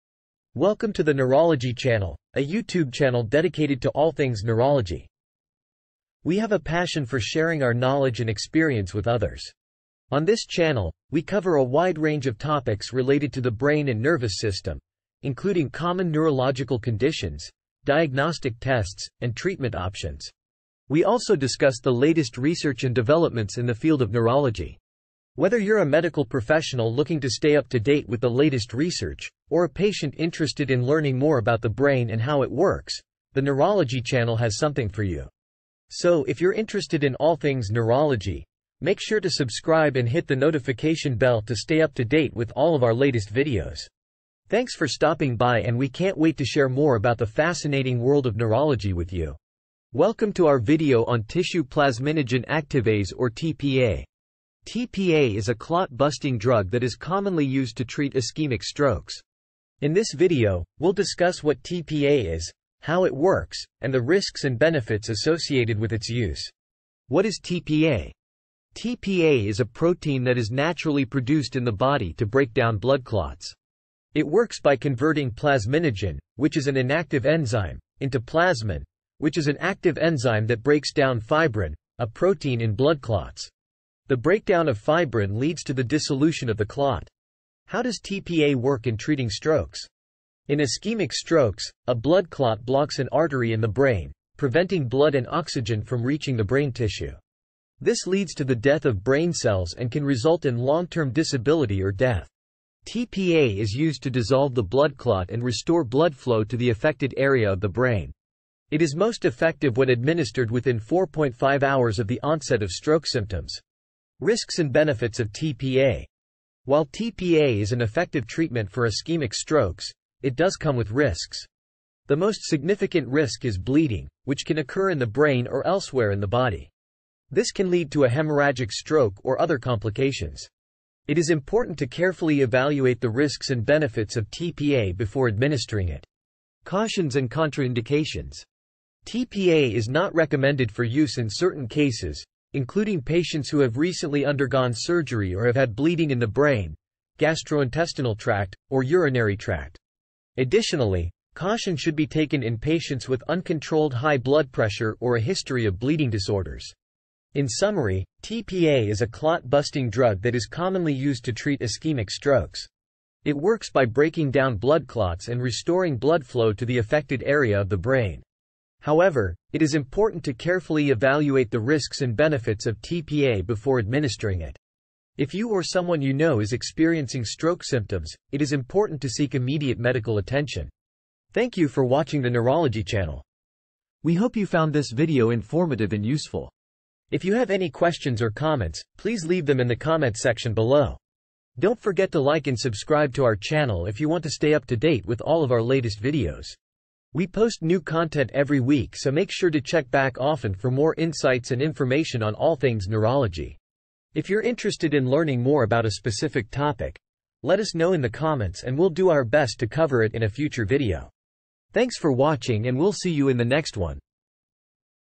All about Tissue Plasminogen Activator (tPA)
In this video we talk all about Tissue Plasminogen Activator or tPA that is used for acute stroke.